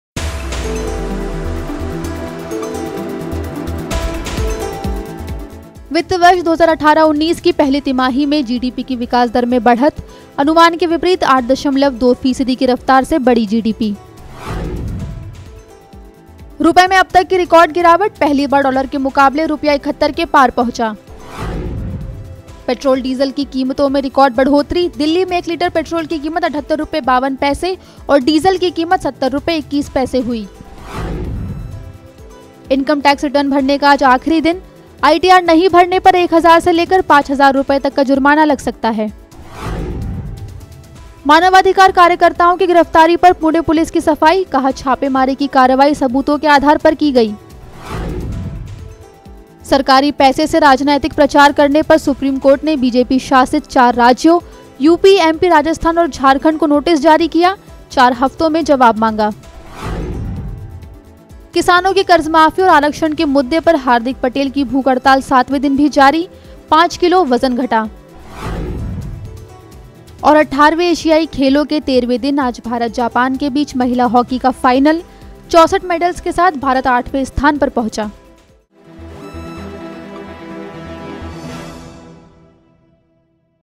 Go Headlines: News In 90 Seconds
GDP grows at 8.2%; Rupee plummets past 71 per US dollar; Fuel price touches record high; Maharashtra Police defends arrests of activists